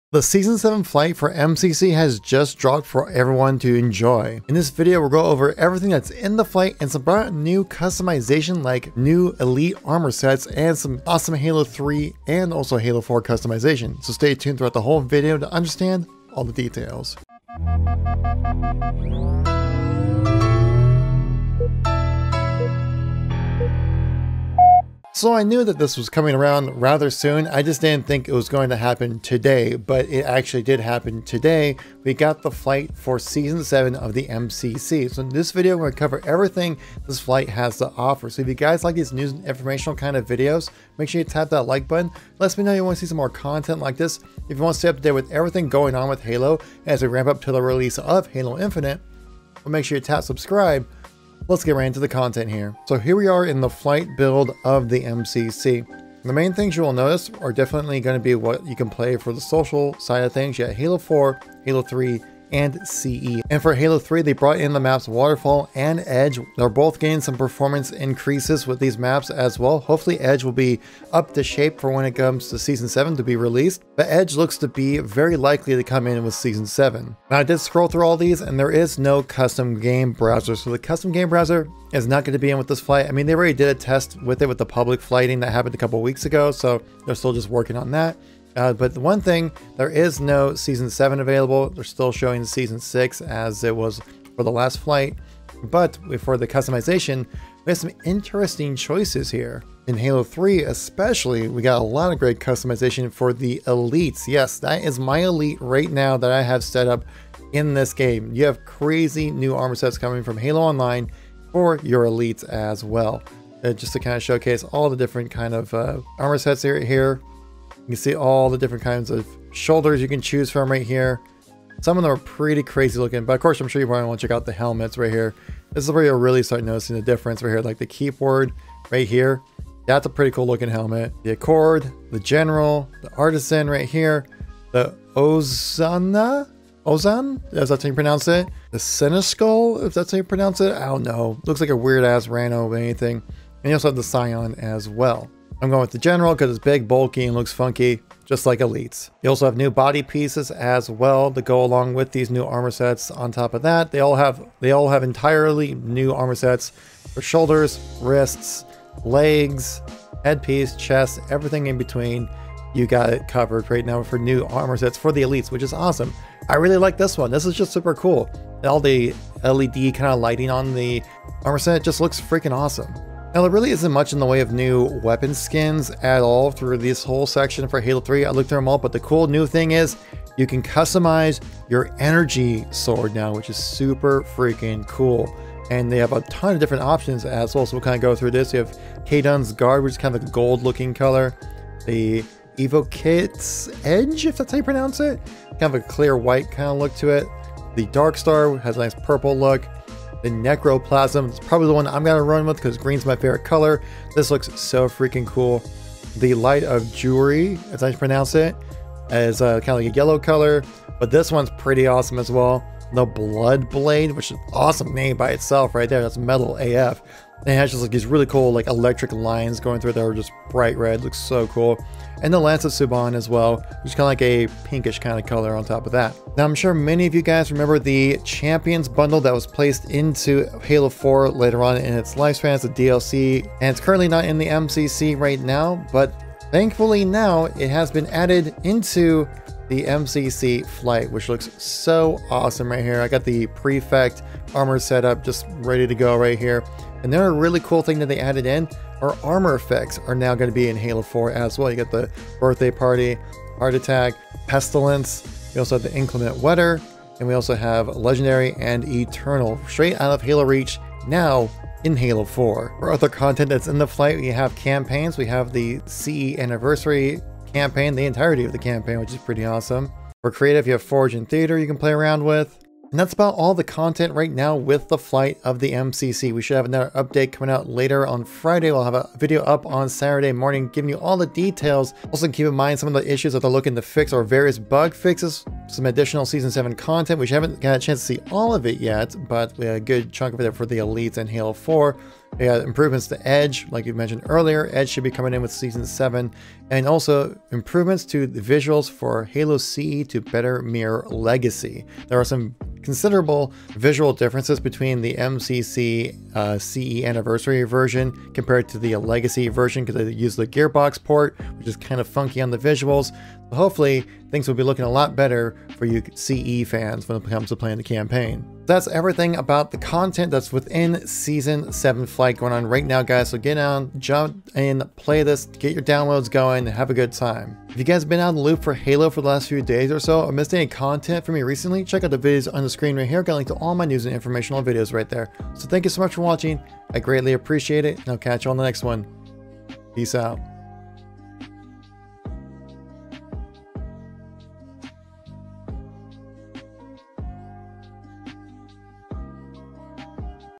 Halo MCC Season 7 Flight is LIVE NOW! New Halo MCC Elite Customization and Halo 4 Champions Bundle!
In this Halo news update video we talk about the recent Halo MCC flight release date. The Halo Master Chief Collection flight has begun for the Halo MCC season 7 flight and it brings new Halo MCC customization for Halo 3 customization and Halo 4 customization as well. Halo Online armor in MCC is coming back but for Halo MCC season 7 armor we have new Halo MCC Elite Armor as well as Halo MCC Season 7 Halo 4 Champions Bundle and Halo 4 armor effects for Halo MCC customization! Halo MCC season 7 release date is still unknown but should be happening within the first half of June.